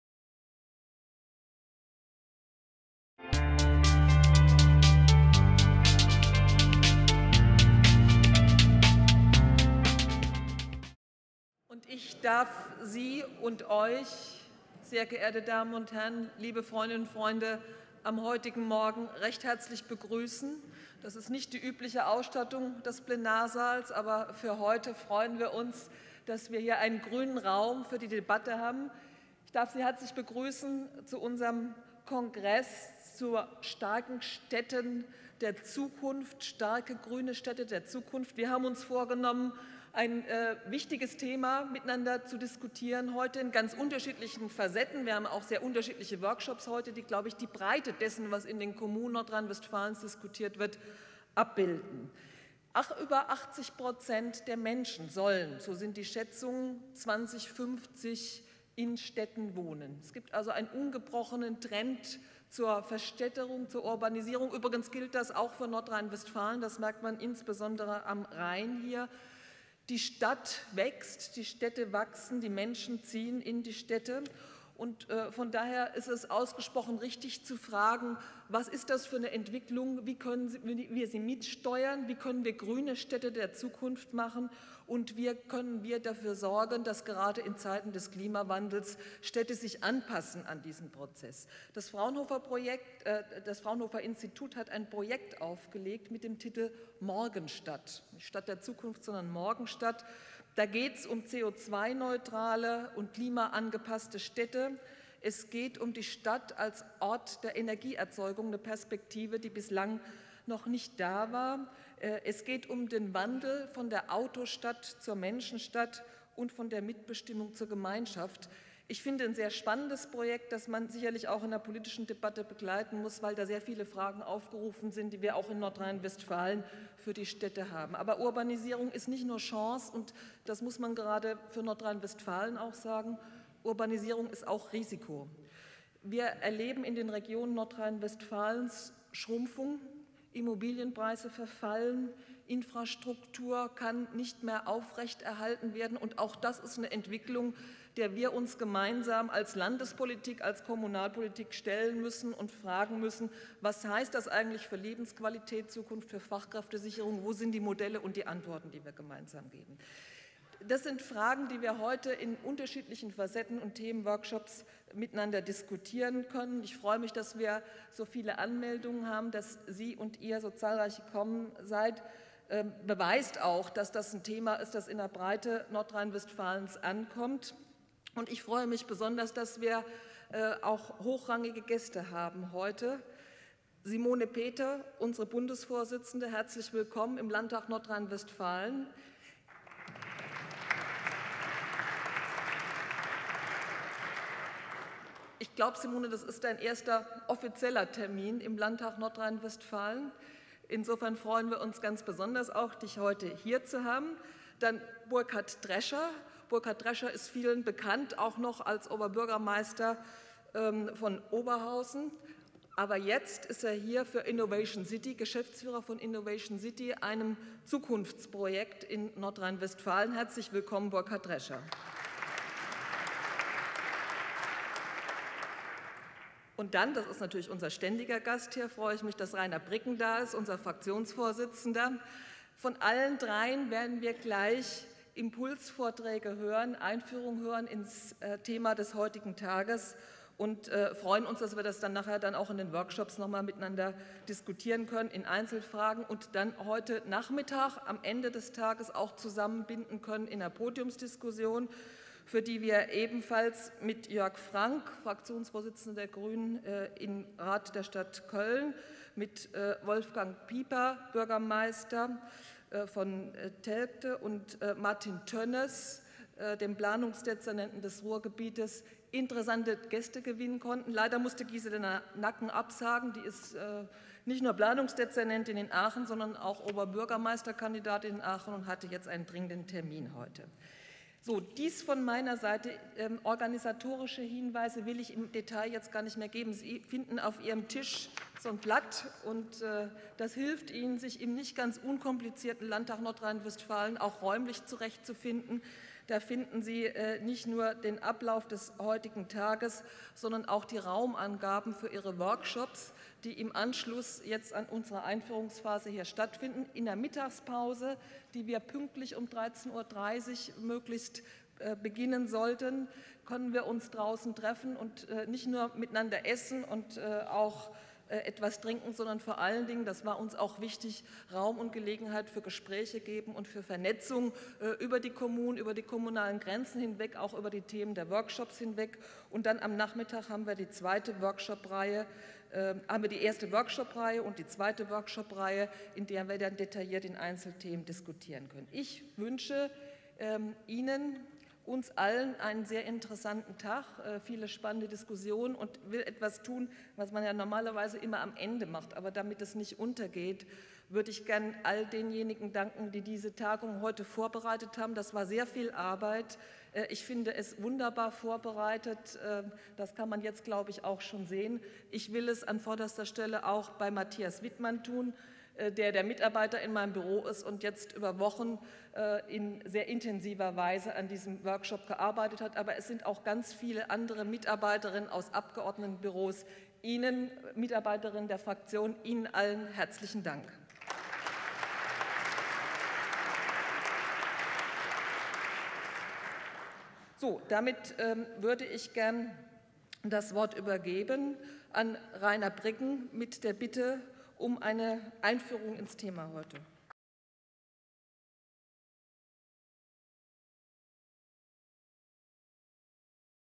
GRÜNE Stadt der Zukunft: Begrüßung Daniela Schneckenburger
Video zum GRÜNEN Stadtkongress im Landtag NRW vom 14. März 2014. Die Grüne Landtagsabgeordnete Daniela Schneckenburger begrüßt die ReferentInnen und über 200 Gäste.